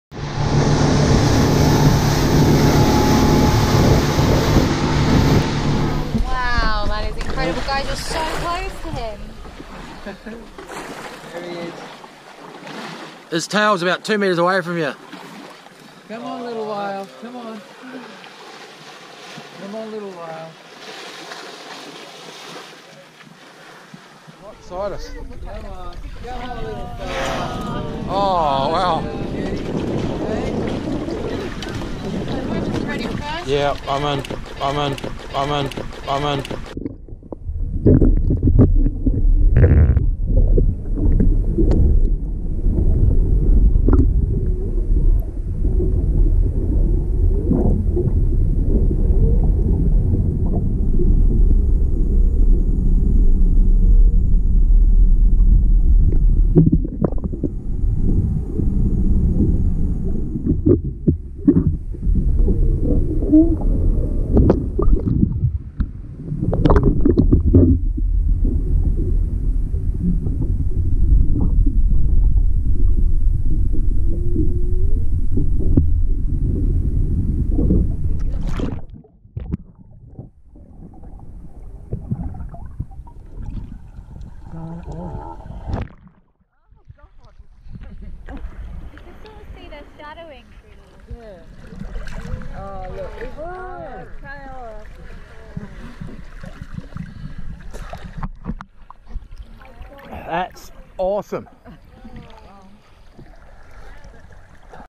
Whale swim and watch, the close version!!
This was an incredible experience, one I will never forget. The whales swimming underneath us are so graceful and curious, best day ever. Totally blew my mind away!